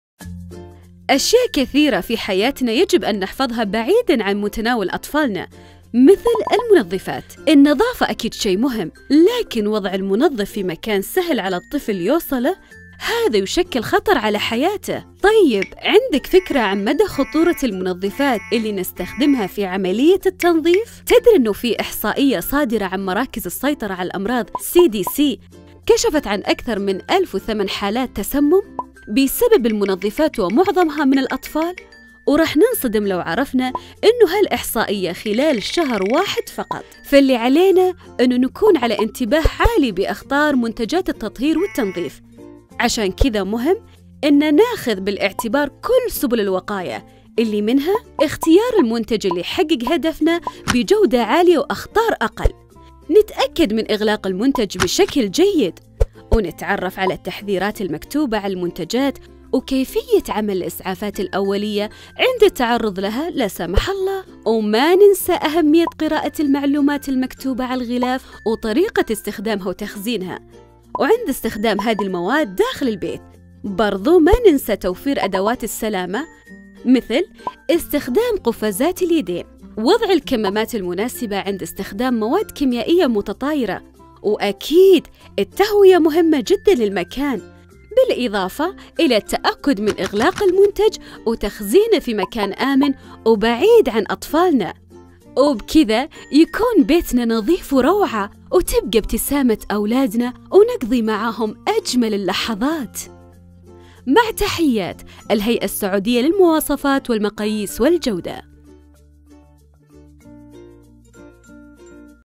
المنظفات المنزلية
كيف تختار المنظفات ذات الجودة الأعلى وطرق التعامل معها؟ هذا ما يجيب عنه هذا الفيلم التوعوي الذي أنتجته الهيئة السعودية للمواصفات والمقاييس والجودة